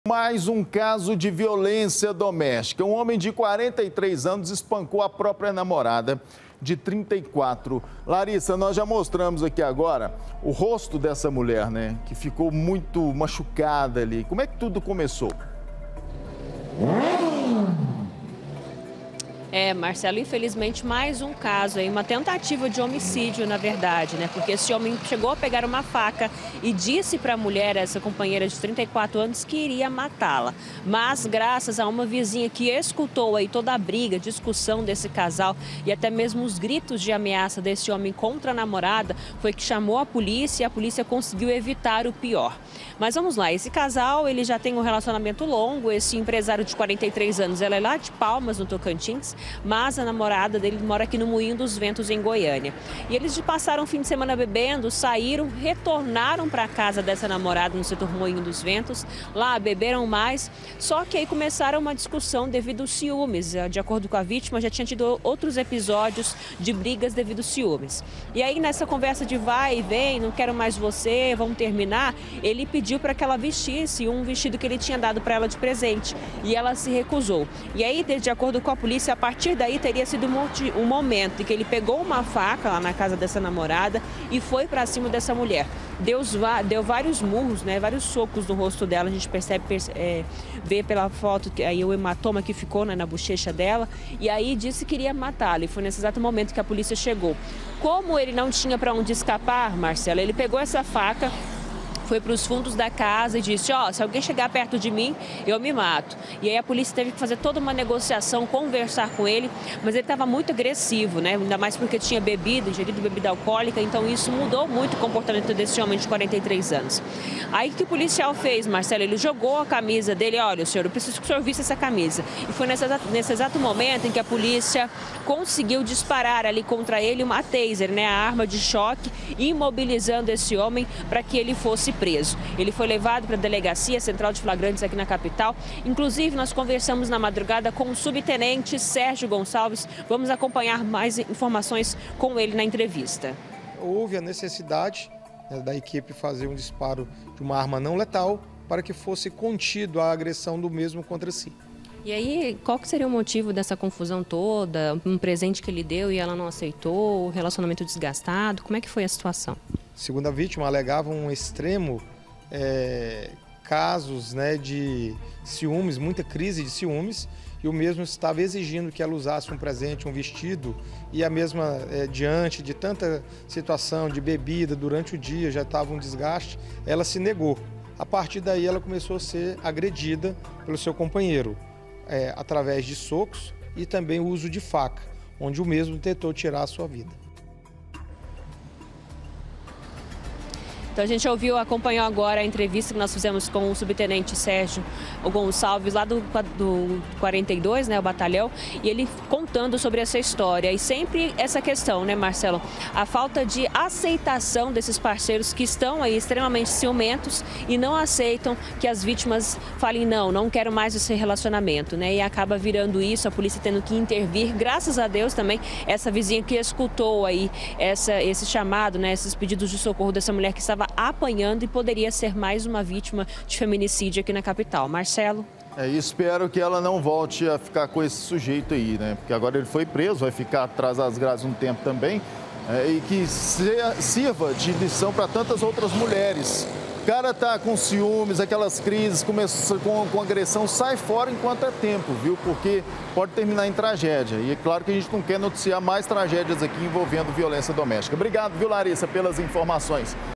BATEU NA NAMORADA: PM USA ARMA DE CHOQUE PARA PRENDER AGRESSOR
A LARISSA CHEGA NOVAMENTE COM OUTRO CASO DE VIOLÊNCIA DOMÉSTICA. UM HOMEM DE 43 ANOS ESPANCOU A NAMORADA DE 34. LARISSA, PELAS FOTOS DÁ PARA VER QUE ELA FICOU BASTANTE MACHUCADA NE?